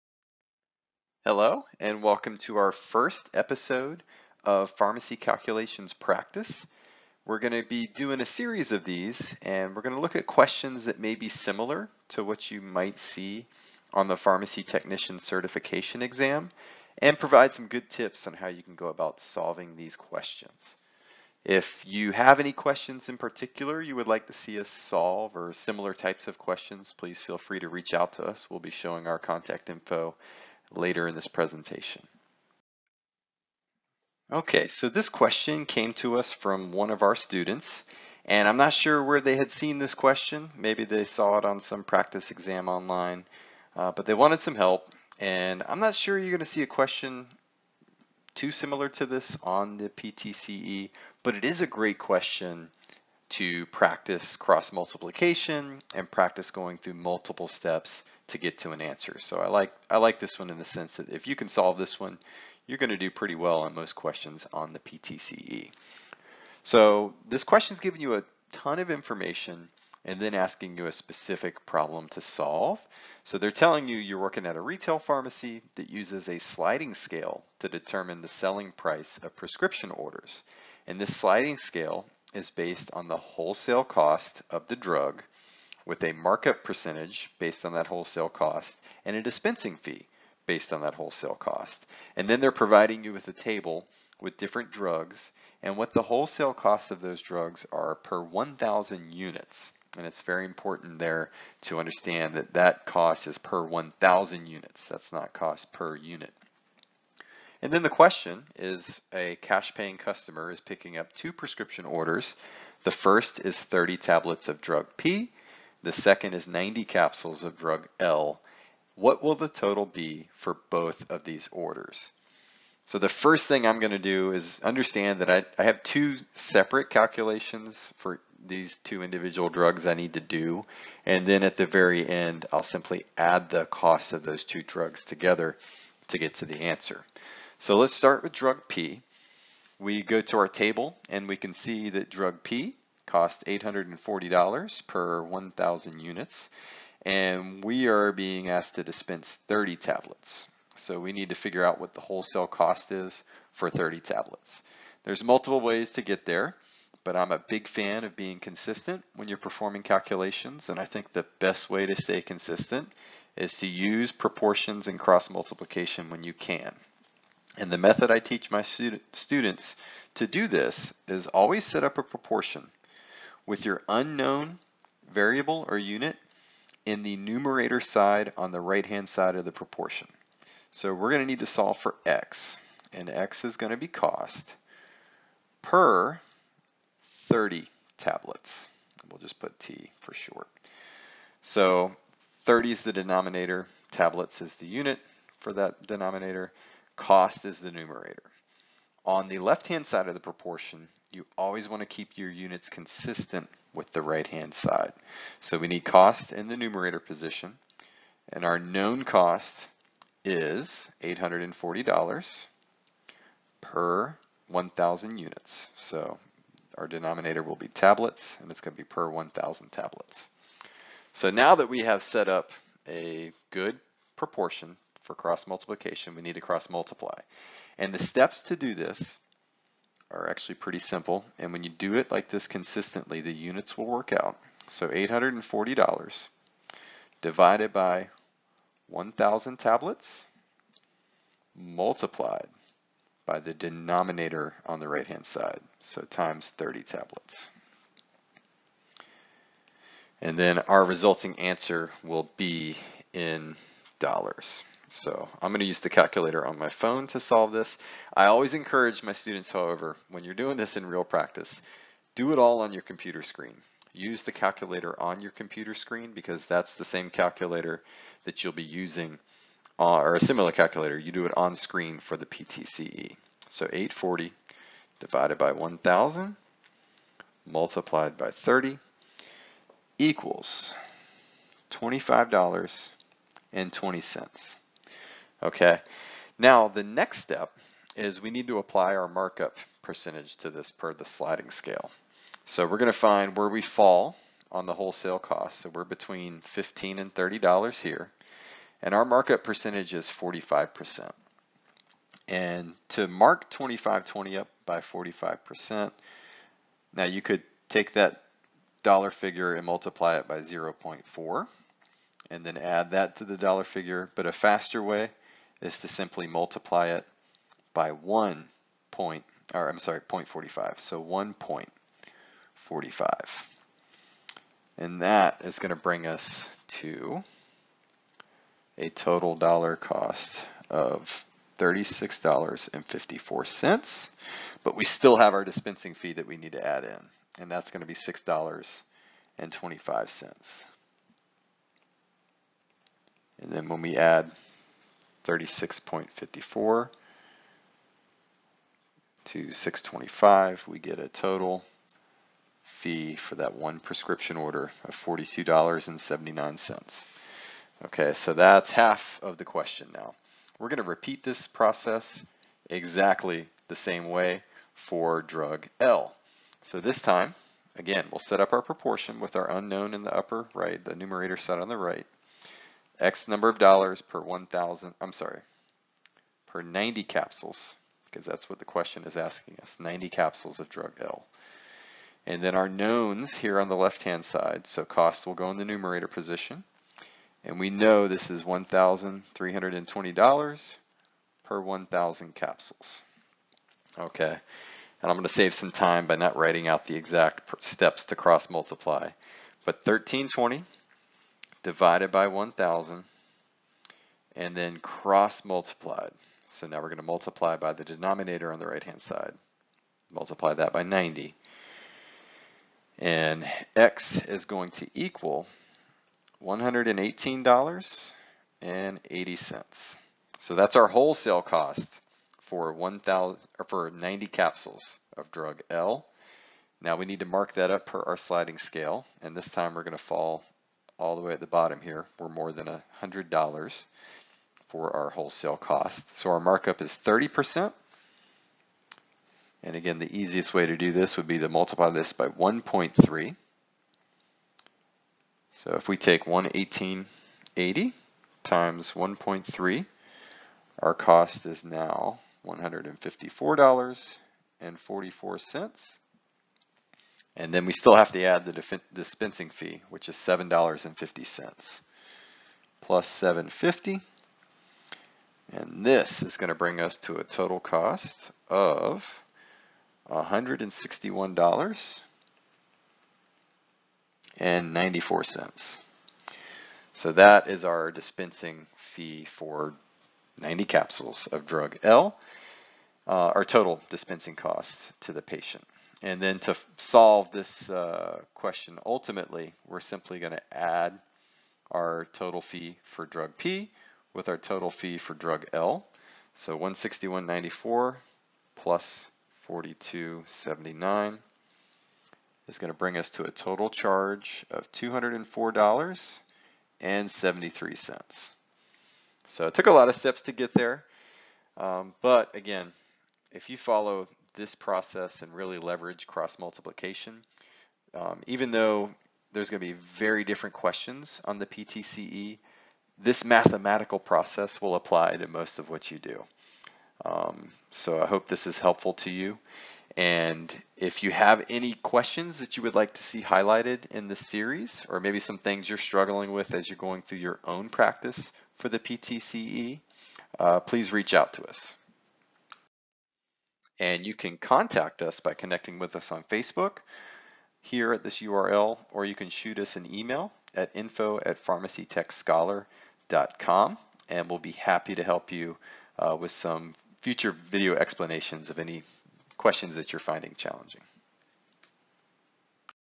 PTCE Pharmacy Calculations Practice: Episode 1
Watch us go through the process of solving a user-submitted question that requires multiple steps and unit conversions. The PTCB pharmacy technician certification exam includes multiple step equations and it is essential to master the art of cross-multiplication and unit conversions to perform well!